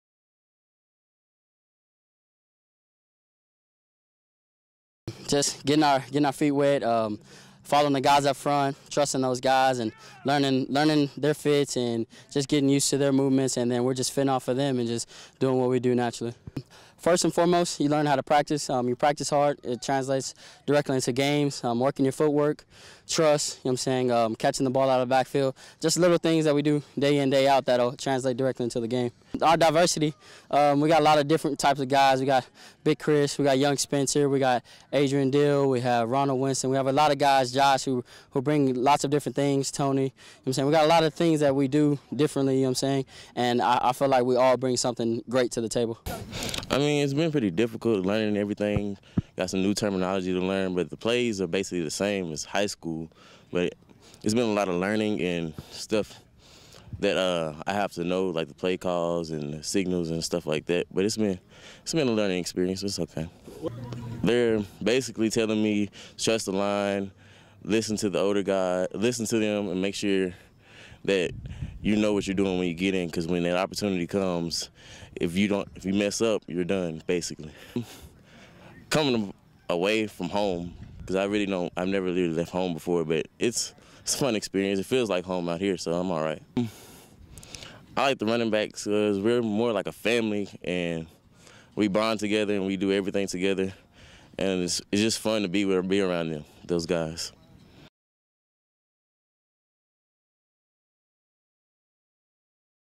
Angelo State Football Fall Camp - Running Backs
Hear from running backs senior Trey Green and freshman Spencer Gilbert II.